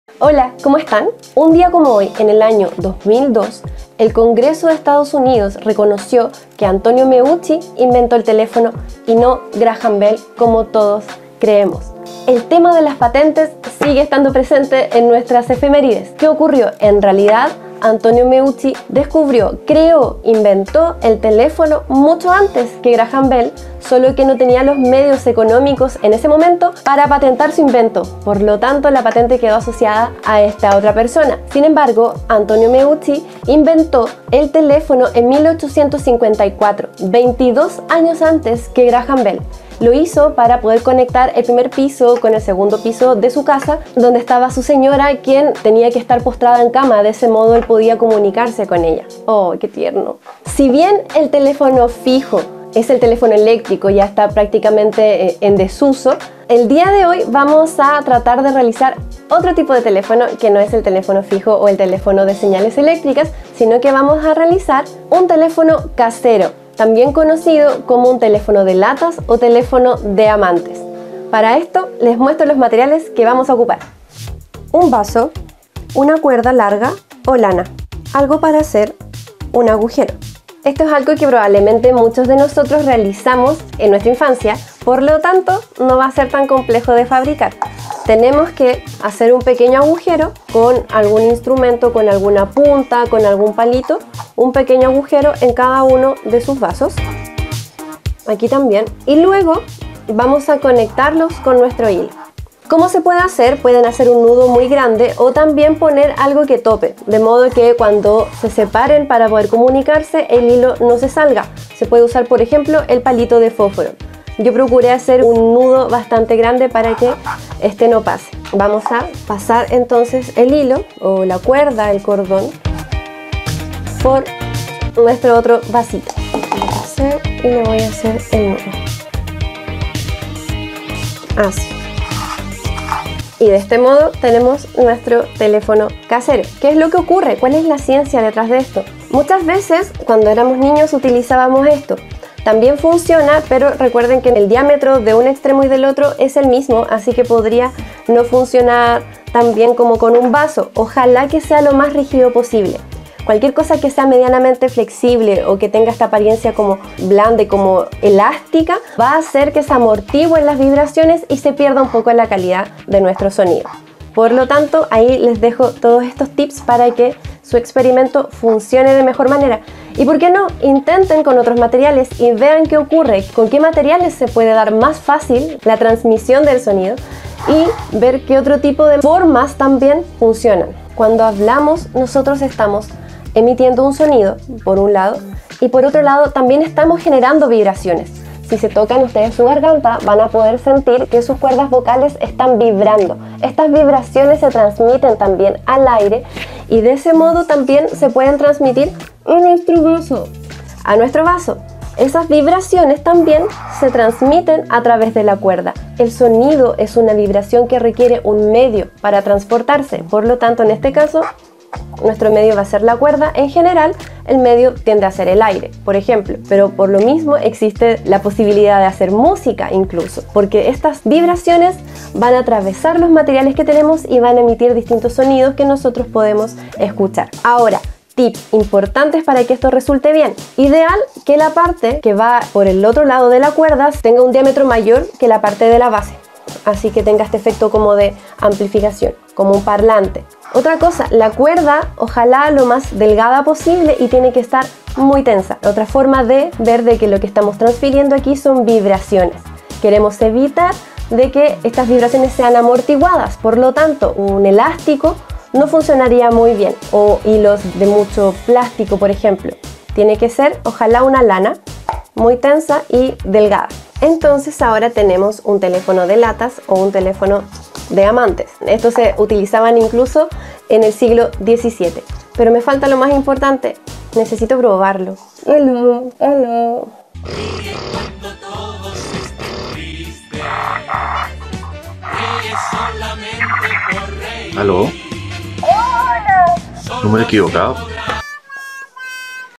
Así se hace un teléfono casero
UnDia como hoy de 2002, el Congreso de EE.UU reconoce que Antonio Meucci inventó el teléfono y no Graham Bell. En esta cápsula nuestra física, Pamela Núñez, nos cuenta esta historia y nos enseña a fabricar un teléfono casero.